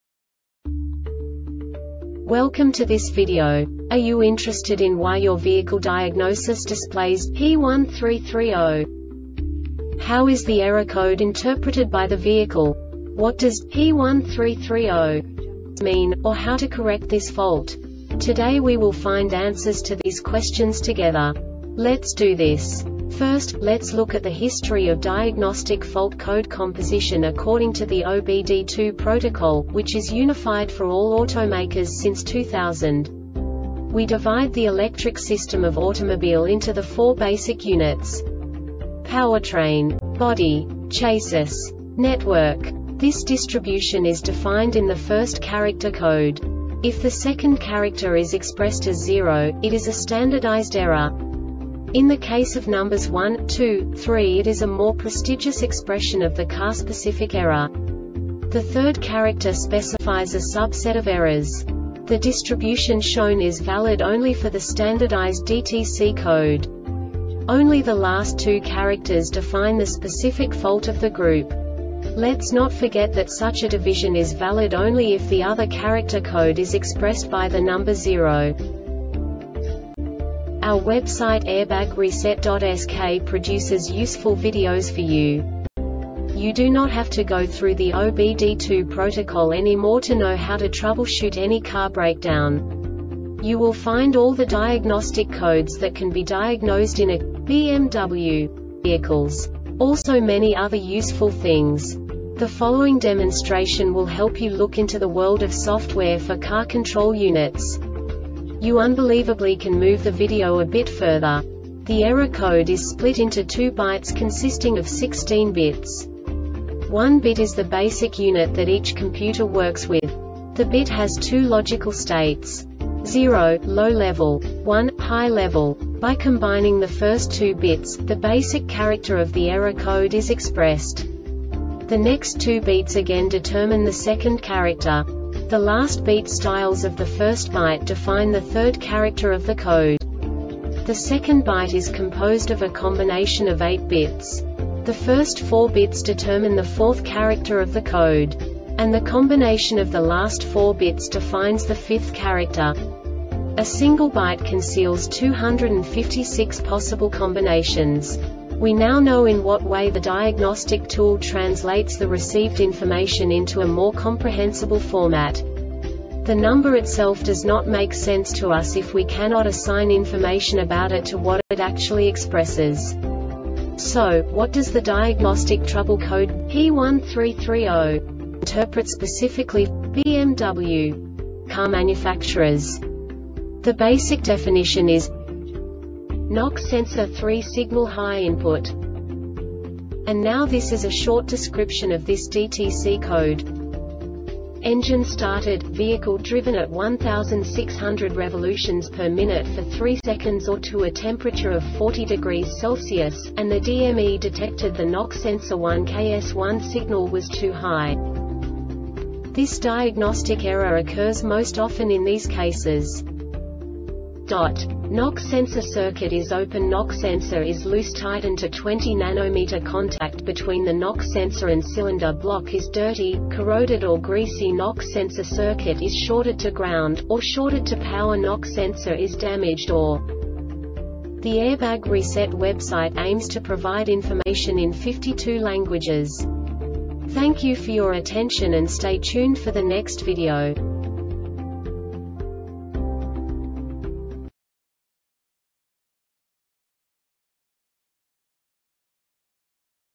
DTC Hyundai P1330 Short Explanation
Contents:
0:21 Basic DTC analysis according to OBD2 protocol standard.
1:48 Insight into programming
2:58 A diagnostically understandable description of the Diagnostic Trouble Code
3:05 Basic error definition

Make
BMW
Code
P1330
Definition
Knock Sensor 3 Signal High Input
Description
Engine started, vehicle driven at 1600rpm for 3 seconds or to a temperature of 40 degrees Celsius, and the DME detected the Knock Sensor 1 KS1 signal was too high
Cause
Knock sensor circuit is open
Knock sensor is loose tighten to 20 NM
Contact between the knock sensor and cylinder block is dirty, corroded or greasy
Knock sensor circuit is shorted to ground, or shorted to power
Knock sensor is damaged or